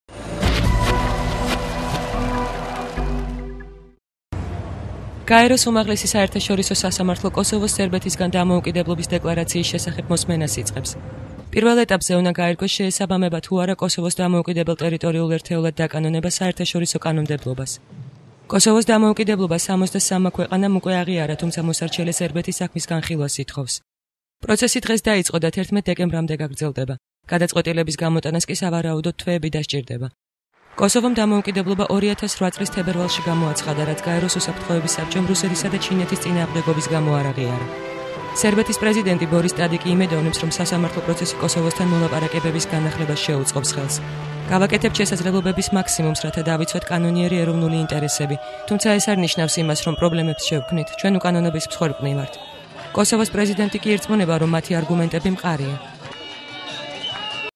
01.12.09
გაეროს სასამართლო კოსოვოს დამოუკიდებლობის განხილვას დღეს იწყებს